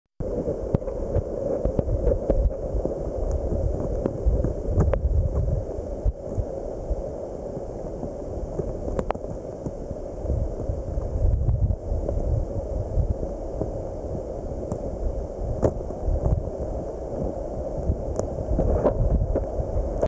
Rasbora pycnopeza from east Sabah (video 1)
Rasbora pycnopeza, (new species of Rasbora from east Sabah; previously thought it was Rasbora sumatrana) is swimming together with Tor tambra.